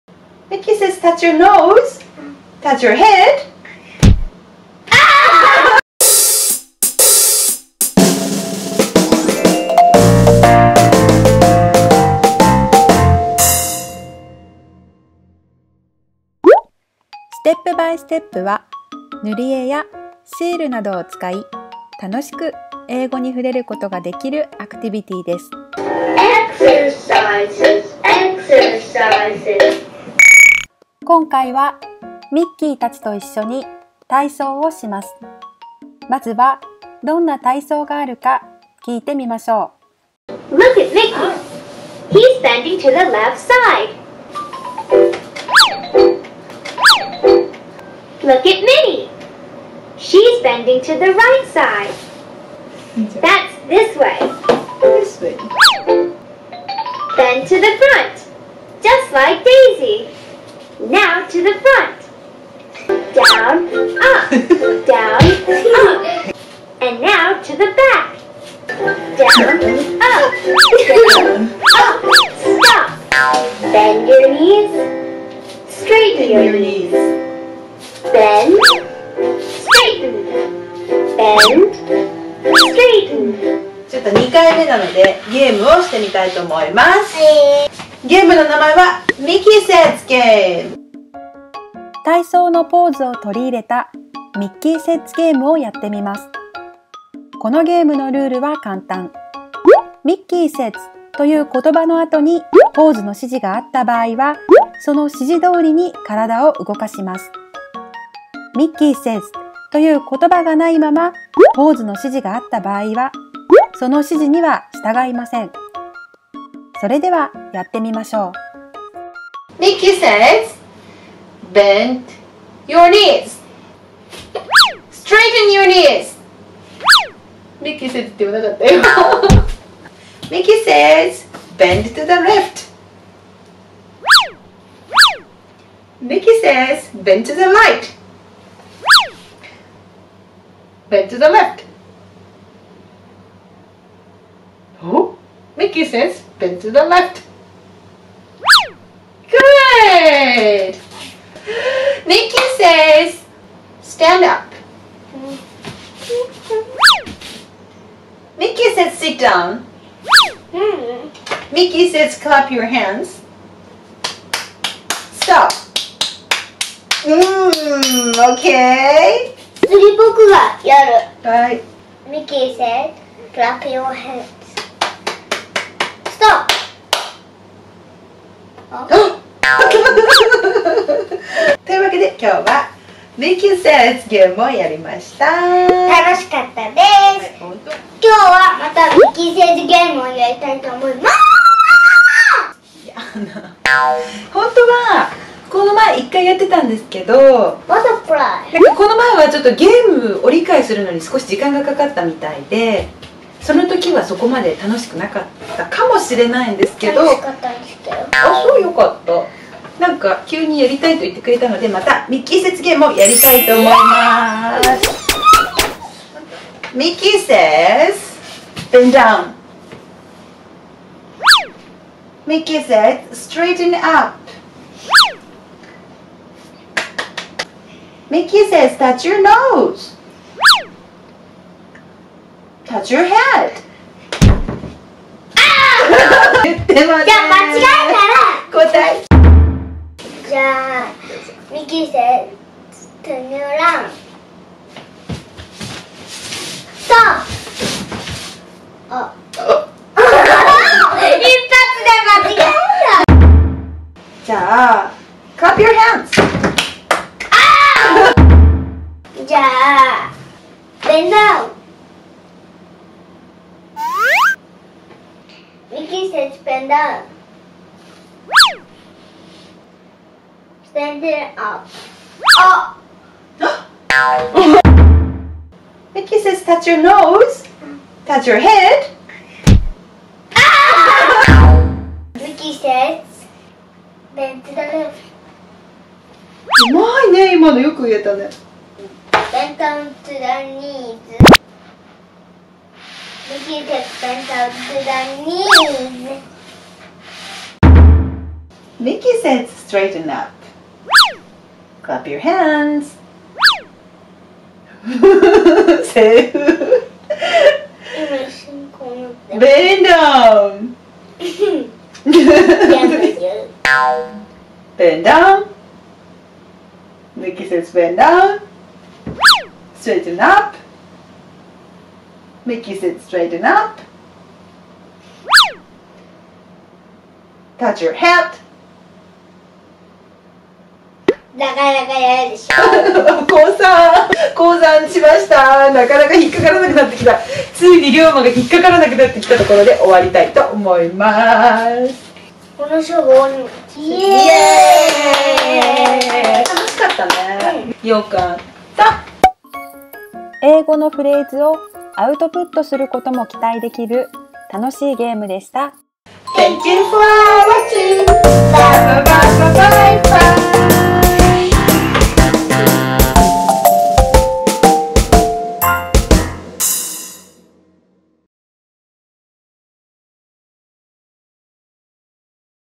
【英語でゲーム】英語のアウトプットには最適！親子で盛り上がるゲーム"Mickey says"！ #117
"Simon Says"という有名なゲームを"mickey"にアレンジした"Mickey
 Says"！体を動かすときの英語表現に慣れ親しんだ後、親子でこの"Mickey says"のゲームをして遊んでみました。指示を出す番になると、英語のアウトプットができ、英語の上達にもグッと近づきます。親子で盛り上がれる楽しいゲームでしたよ！

●5歳で海外英会話デビュー！"ディズニーの英語システム"Blue CAPをとってハワイに行ったら！ ？ Went to Hawaii after getting a Blue CAP! #80